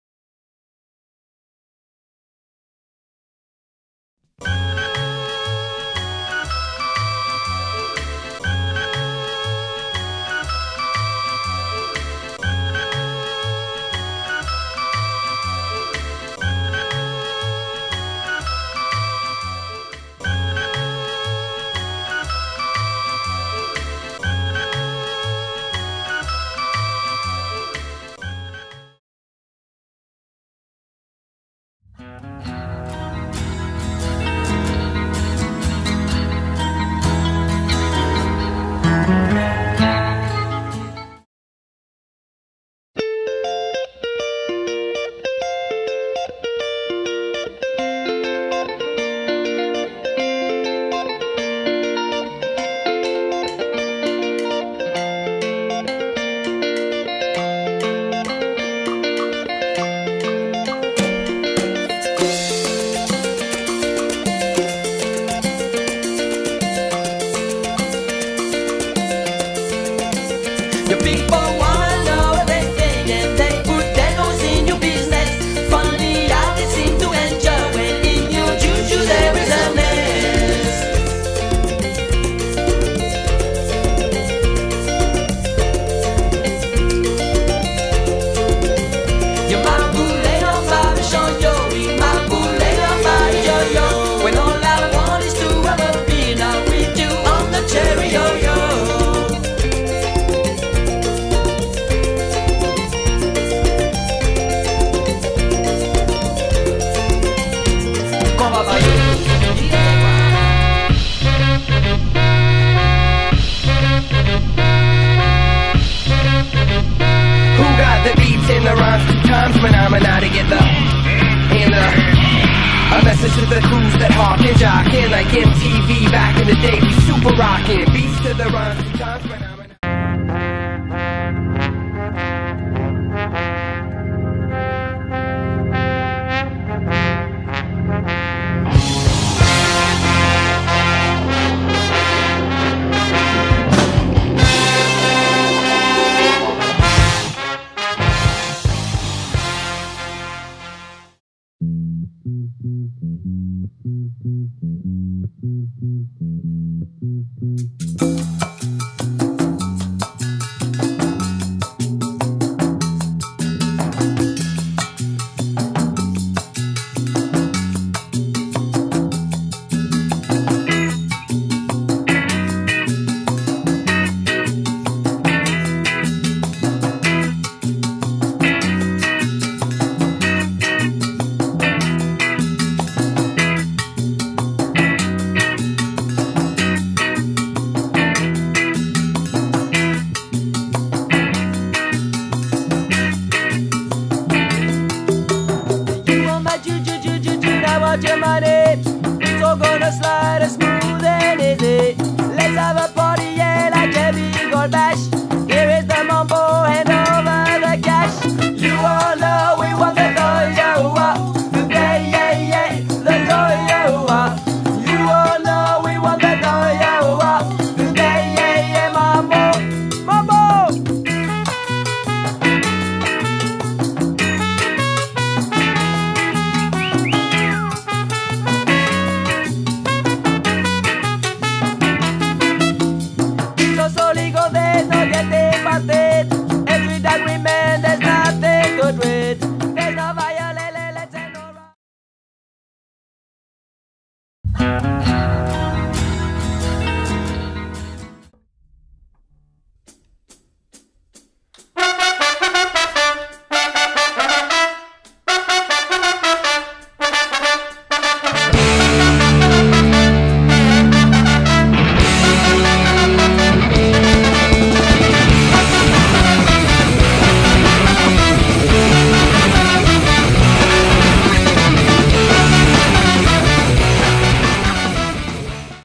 The Gentleman's Quarrel
Two dorm slobs see the most beautiful girl and they decide to spruce up and impress her, but the outcome is not what they expected...
Special thanks to Campus Movie Fest for the all the equipment and music used to make this video.